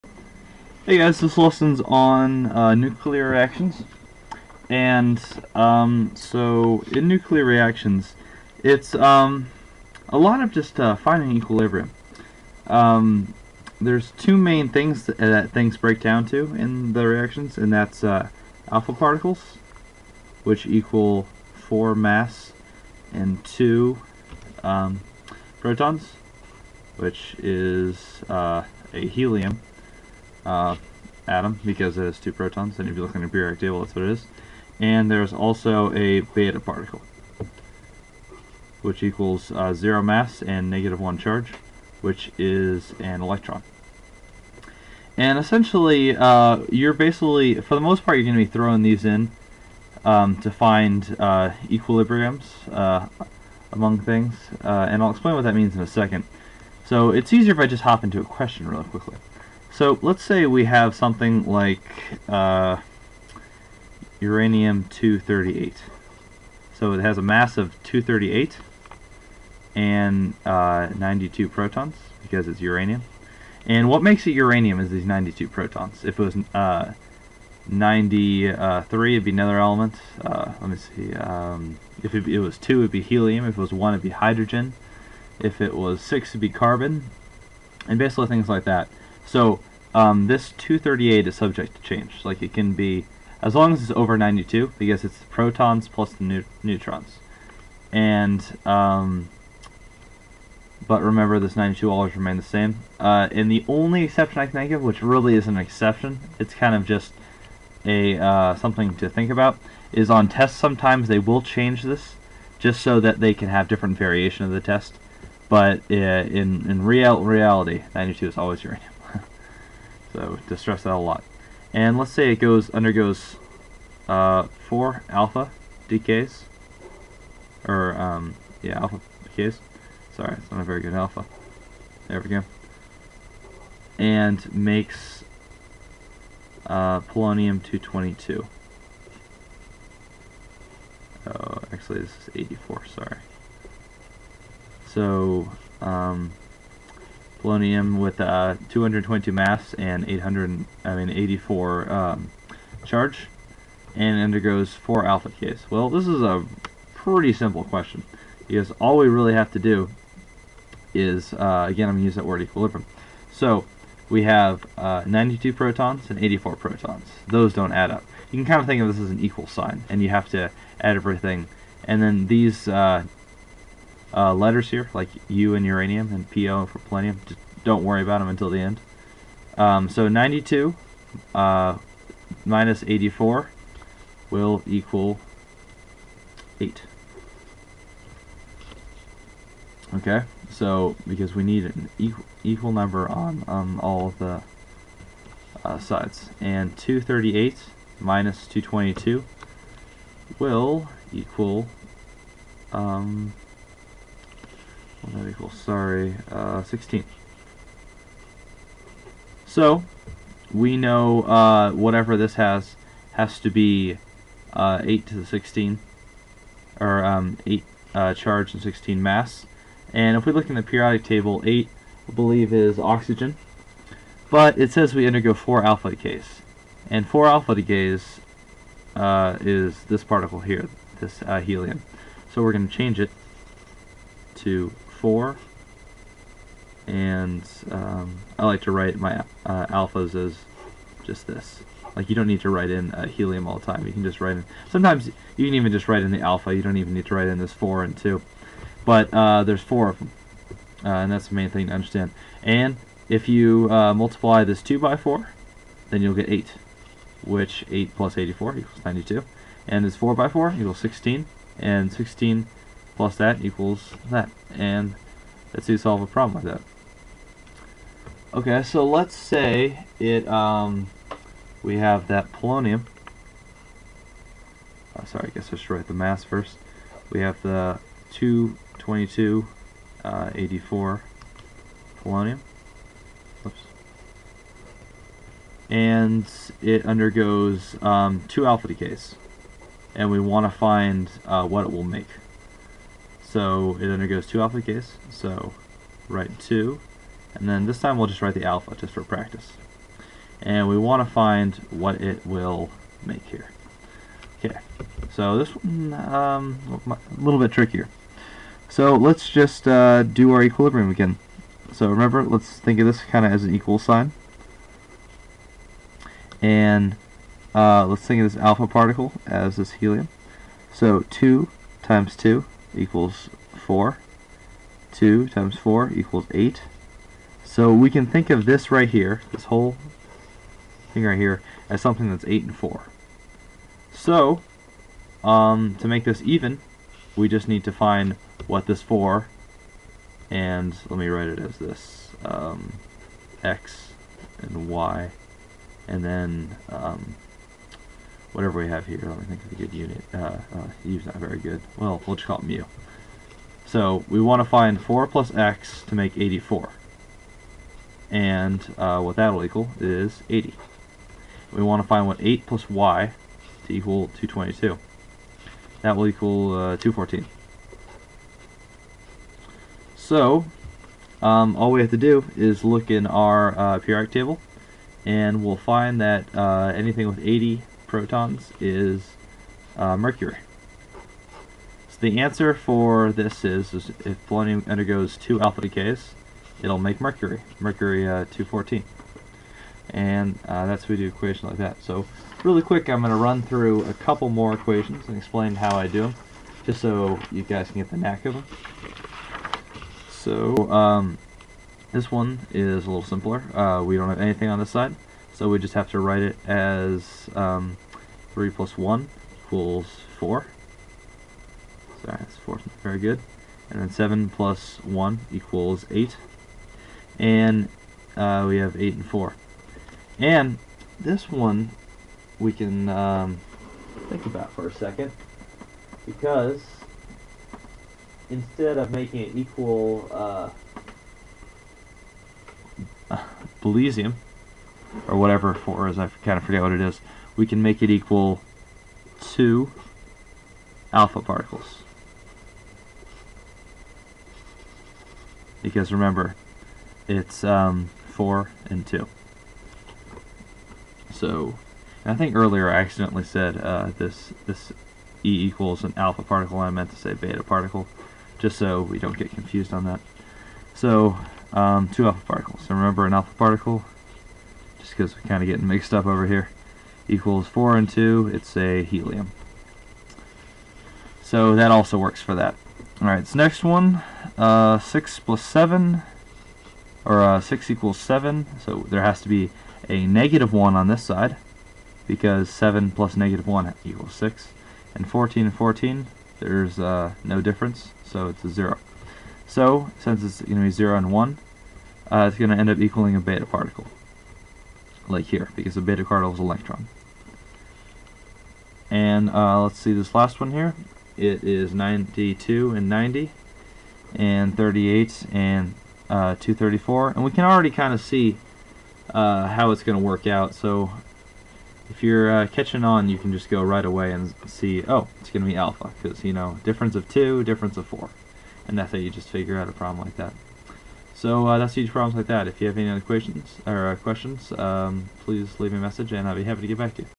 Chemistry Nuclear Reactions
How to solve nuclear reactions and equations using alpha and beta particles from different radioactive isotopes like uranium.  Uranium Isotope 238/92 U transformes to 5 consecutive α - decay polonium 218/82 nucleous(nuclei).  #1 What types of nucleous or particles we have to "bomb" the nucleoses, in order to happen following nuclear reactions